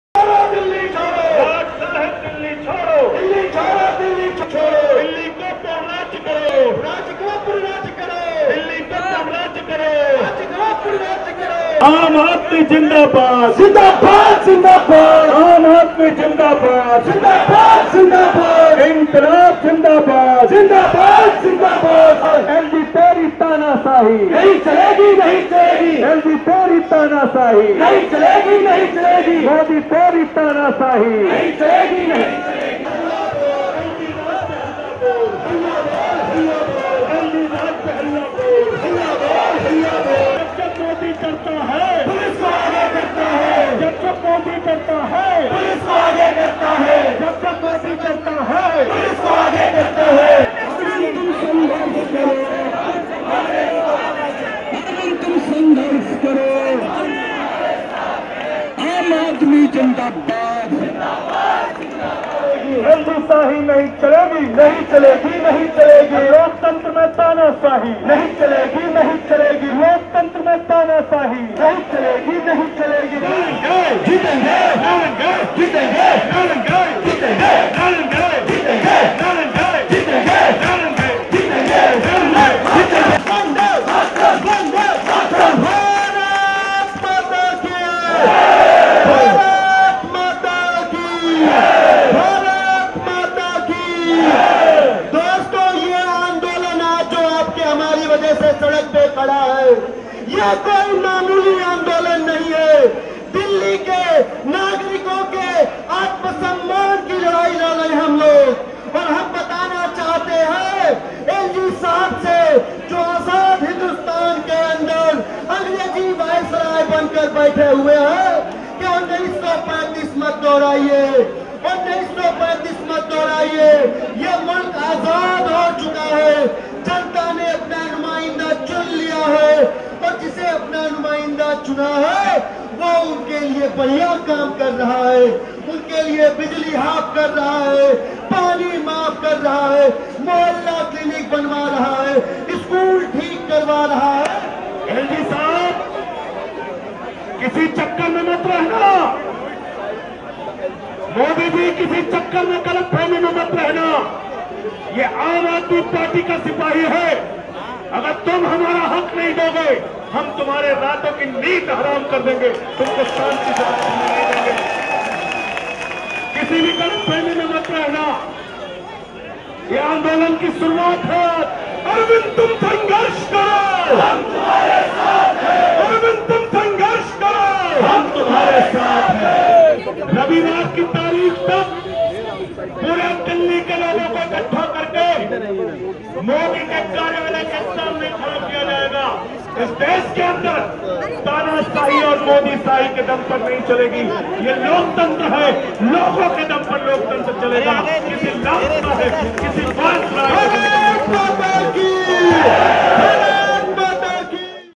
AAP Solidarity March to Support the Demands of CM (JUNE 2018)
Aam Aadmi Party leaders and workers have gathered outside Lieutenant Governor Anil Baijal's office, where Chief Minister Arvind Kejriwal and his ministers have been on a sit-in protest for 48 hours. They were joined by former Union minister Yashwant Sinha, who resigned from the BJP recently. Rajya Sabha member and senior AAP leader Sanjay Singh has said that if the matter has not been resolved by Sunday, they will march to the Prime Minister's Office. AAP claims it has the support of other opposition parties in their protest.